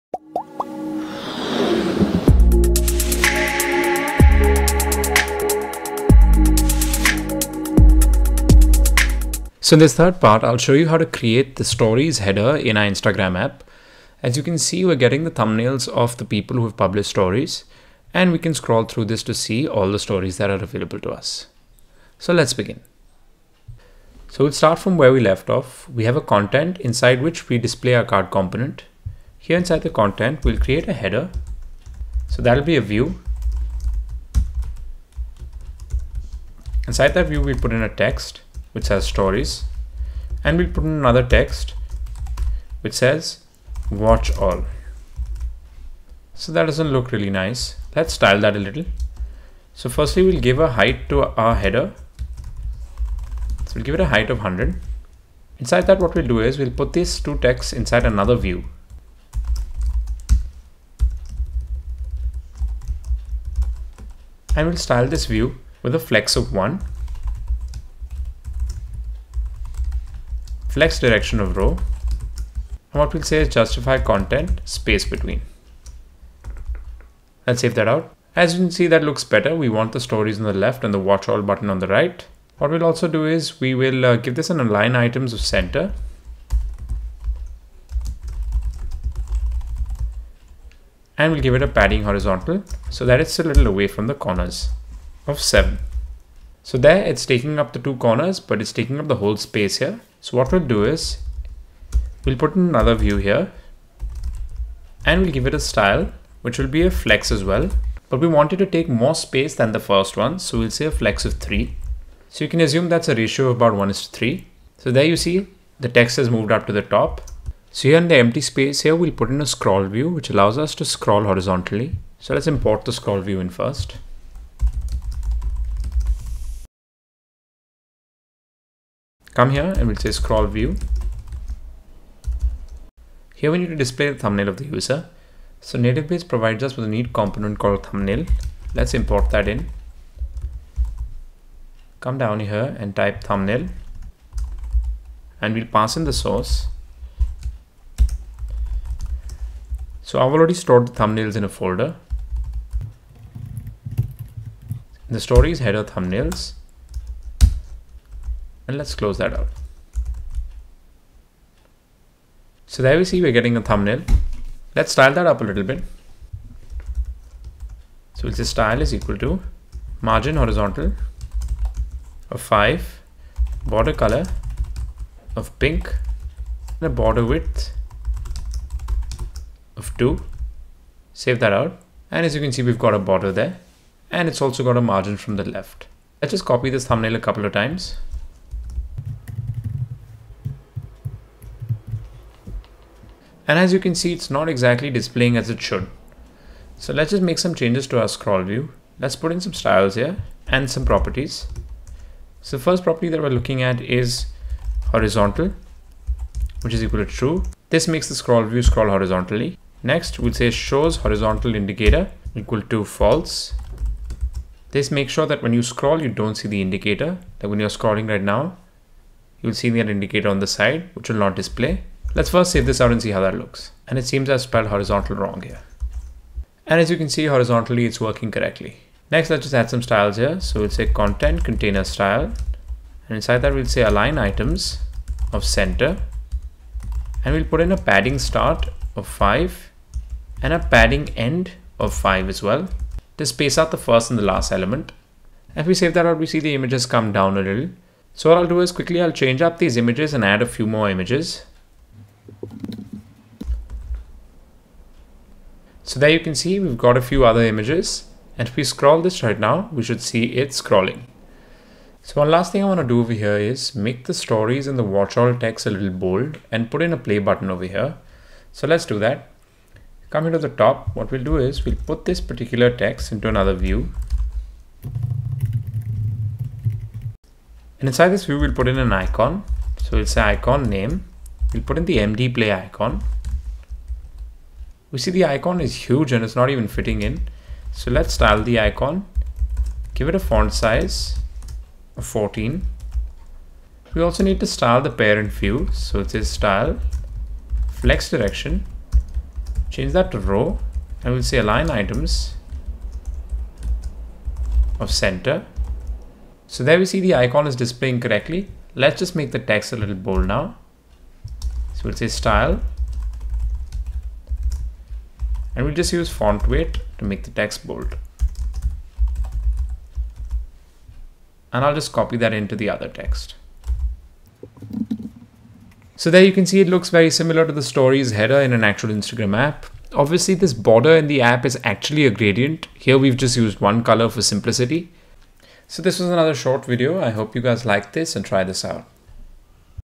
#3 Instagram Stories UI Tutorial | React Native Layout(UI) Series
#3 - Build the UI of the Instagram Stories Header using React Native ScrollView and Native Base

New To React Native?